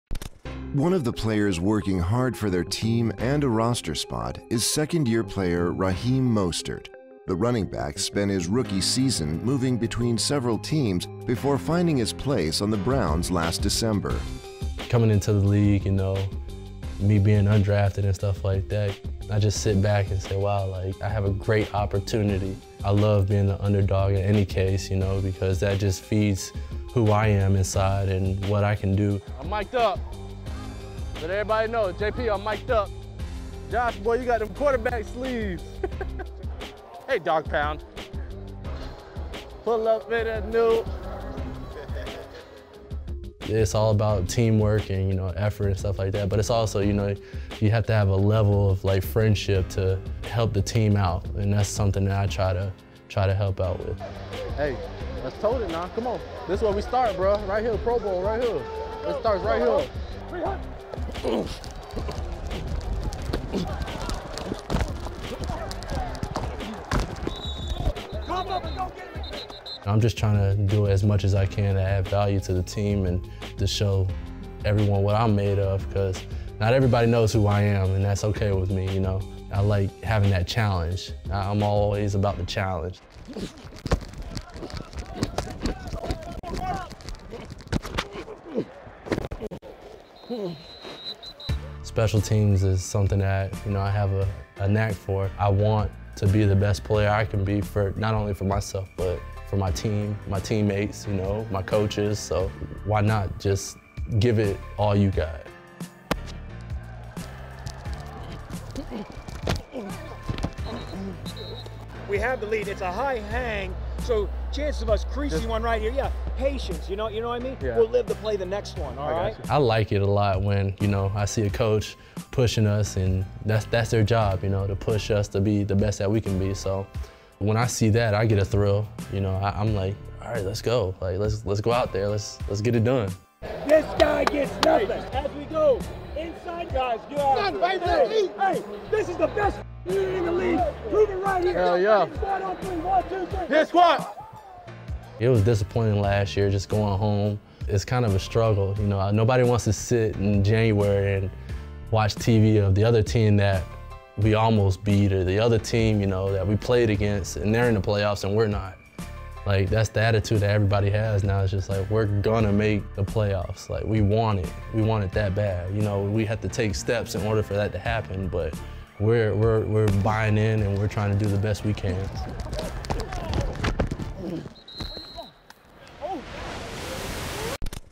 Raheem Mostert pushing for roster spot
Second year running back Raheem Mostert has been showing sings of improvement with his speed and agility during training camp. Mostert was mic'd up during the Browns' second pre-season game against the Atlanta Falcons.